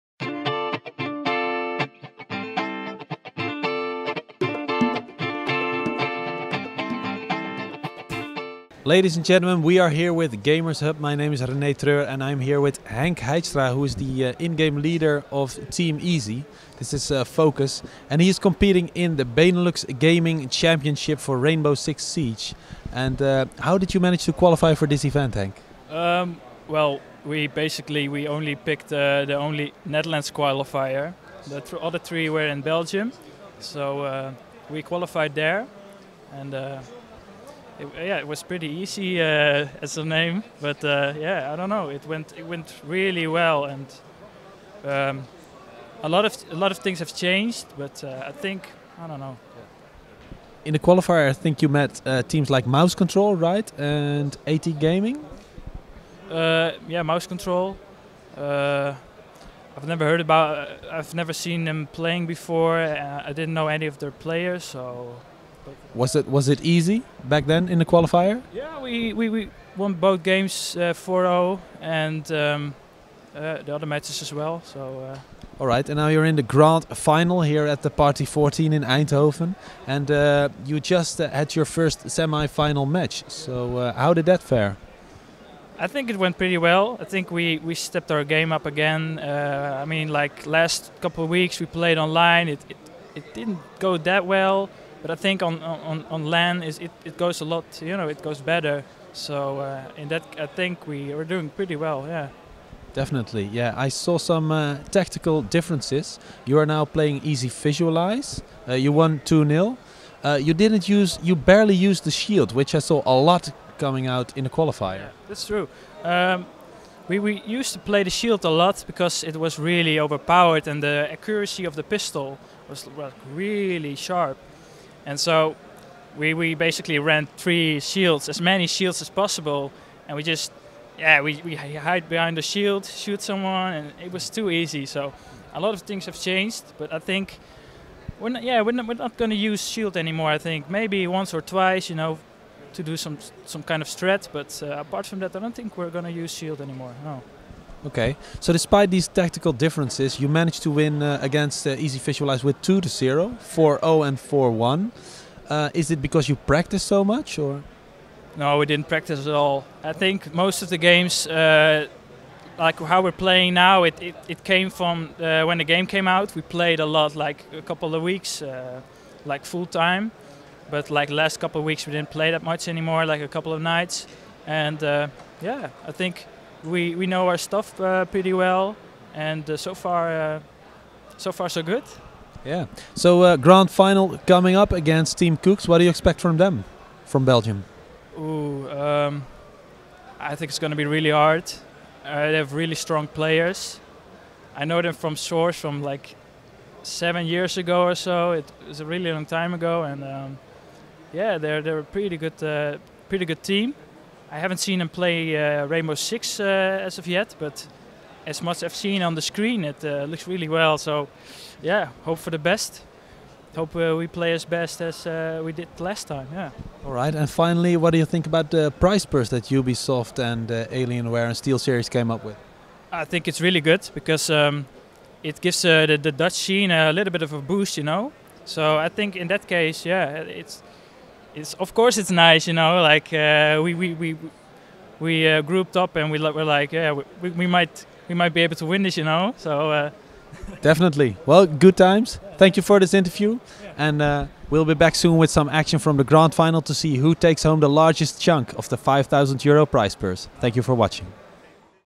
The Party 14 | Interview Henk Heistra Rainbow Six Siege
In this video:
Henk Heistra shares his experiences on the Rainbow Six Siege Ubisoft competition at The Party 14

Interviewee:
Henk Heistra

Host:
Rene "LiQuidmoTion" Treur